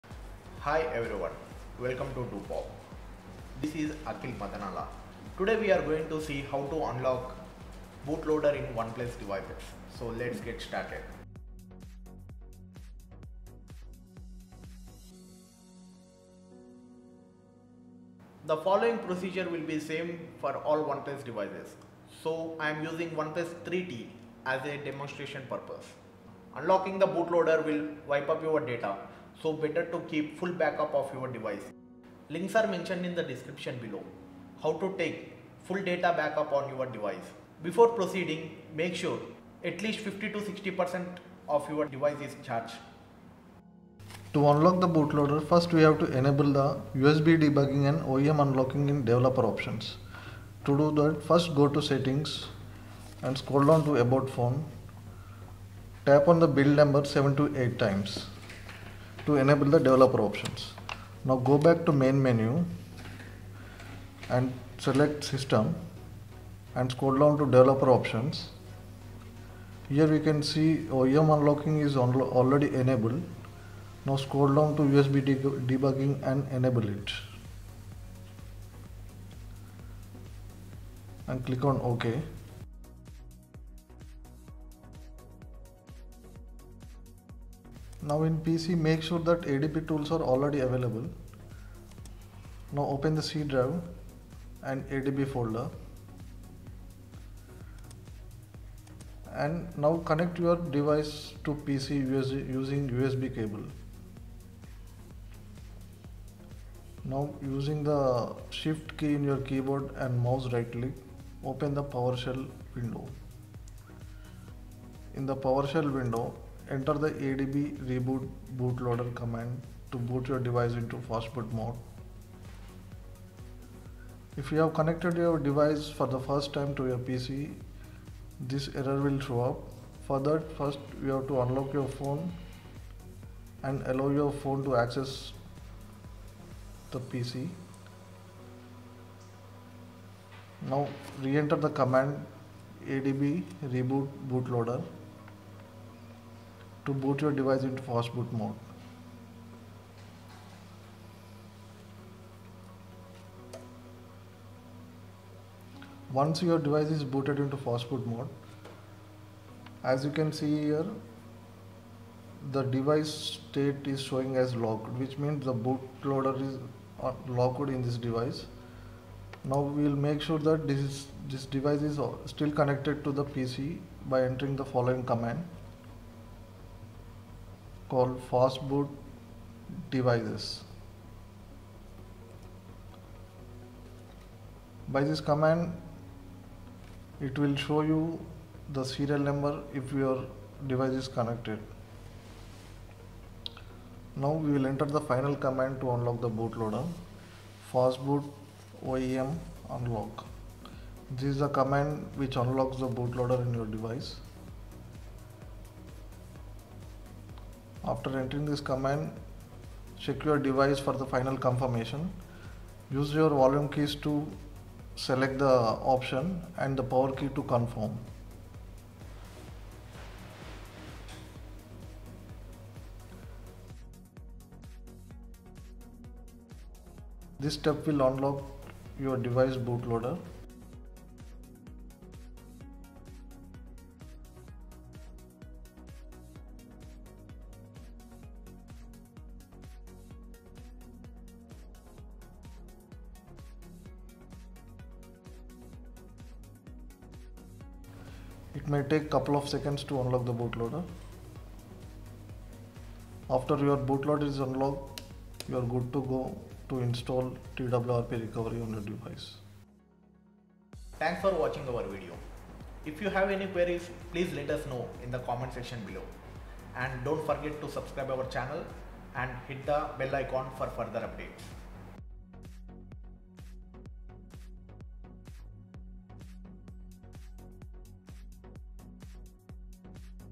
How to Unlock Bootloader In Any OnePlus Device
How to Unlock Bootloader in OnePlus Devices

Supported Devices:
OnePlus 8 Pro
OnePlus 8
OnePlus 7T Pro
OnePlus 7T
OnePlus 7
OnePlus 6T
OnePlus 6
OnePlus 5T
OnePlus 5
OnePlus 3T
OnePlus 3
OnePlus X
OnePlus 2
OnePlus One